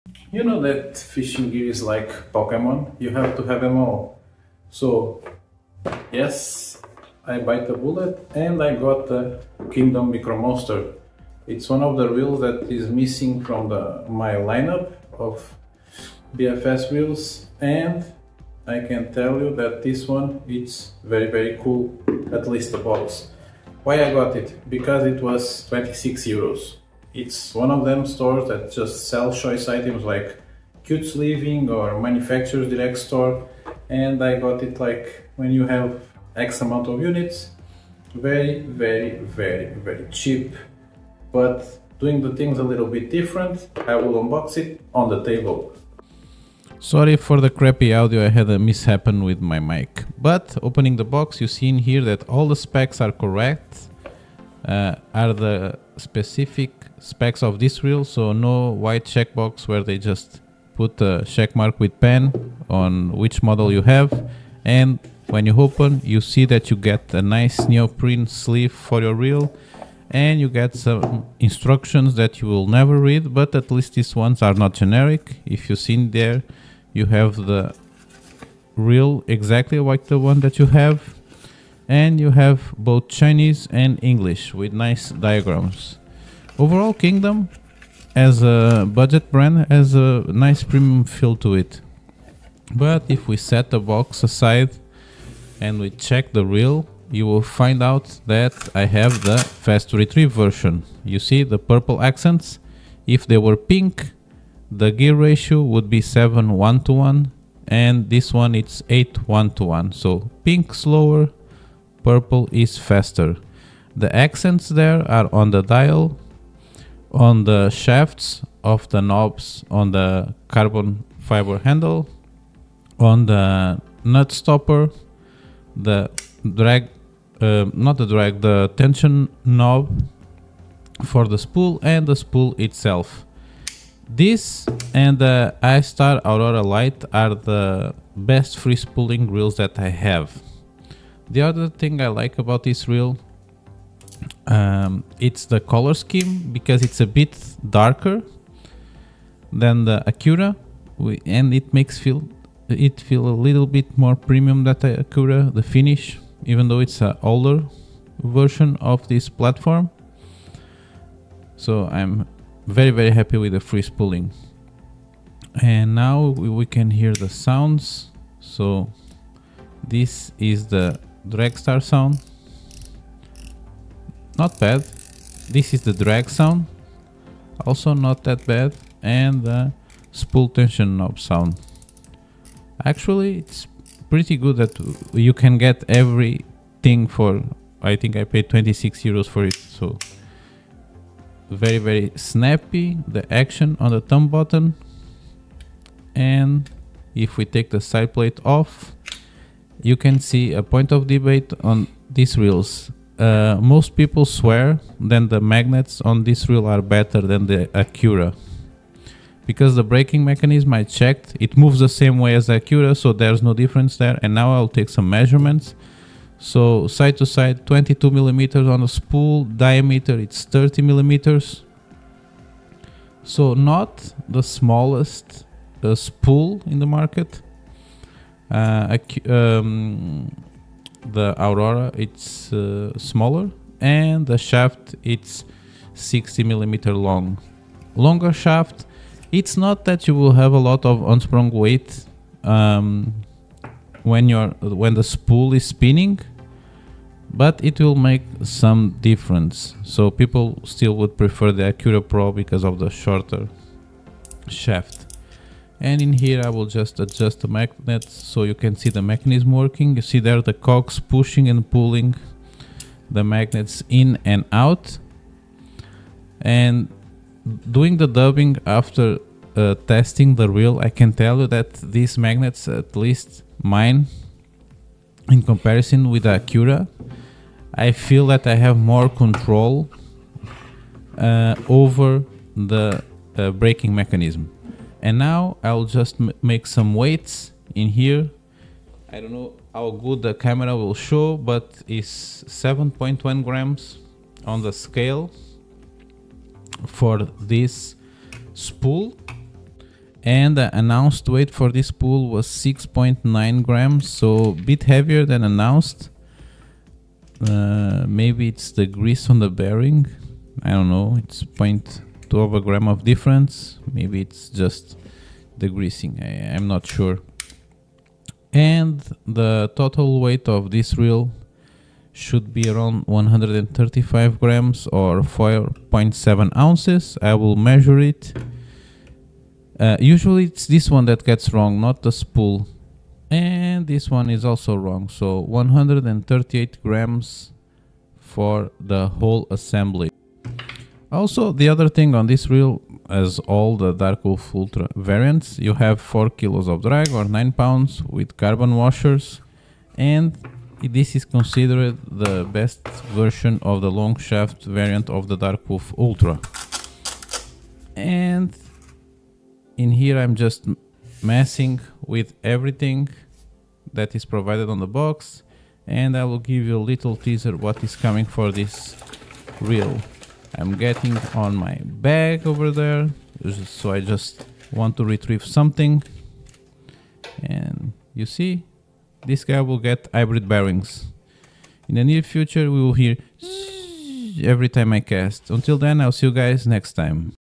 Kingdom Micro Monster - Still a good deal?
Hello everyone!

I just unboxed the ultralight Kingdom Micro Monster casting reel!  This BFS (bait finesse system) champ weighs in at a crazy light 135 grams (4.8oz) and it boasts features perfect for BFS enthusiasts:

-Carbon fiber frame for effortless casting, high-strength aluminum main gear for durability. (BFS reel, lightweight baitcasting reel, finesse fishing reel)

-Choose 7.1:1 or 8.1:1 gear ratio and two spool with the shallow coming in at 6.9grams!

-Long Casts, Controlled Fights: Magnetic braking system for exceptional casting distance and controlled retrieves.

-"Smooth" Performance for the price: 8+1 bearings for steady retrieves, even under pressure.

Kingdom Micro Monster